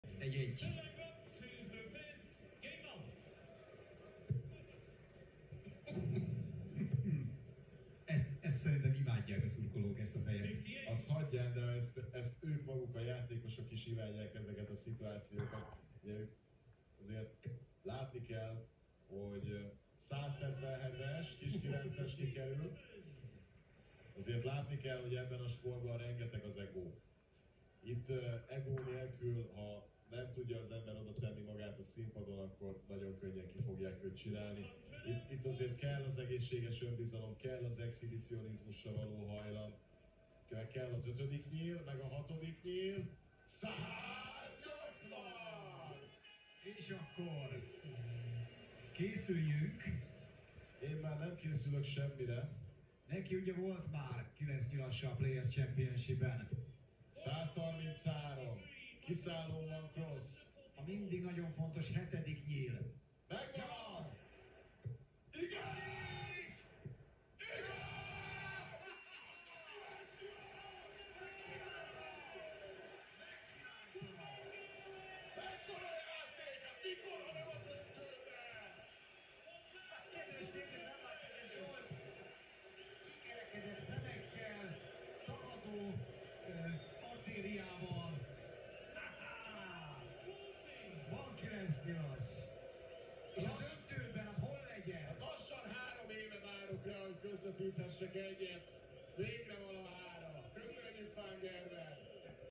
MvG 9 darter at Players Championship Final 2022
Michael van Gerwen hit a 9 darts finish at the PDC Players Championship Final's final.
27/11/2022 in Minehead / England.

Hungarian commentaries.